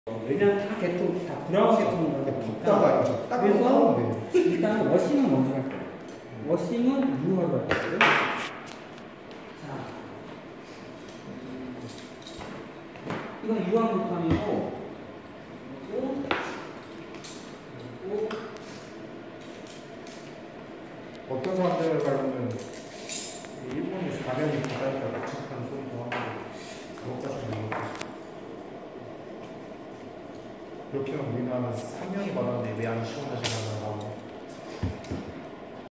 튜닝타임즈 프라모델 강좌 용산팀 김동환군의 Bandai 반다이 RG 1/144 Unicron Gundam 유니콘 건담
용산 아이파크 문화센터 이상백 강사님의 프라모델 디오라마 수업 수강생 김동환군이 밑색을 깔고서 음영이 은은하게 올라올 수 있도록 명암 색칠을 해줬는데요, 빨강과 노랑, 파랑의 색상으로 구성된 퍼스트 건담처럼 컨셉을 잡고서 색칠해줘서 기존의 유니콘 건담과 뭔가가 달라 보입니다^^ 프라모델 튜닝타임즈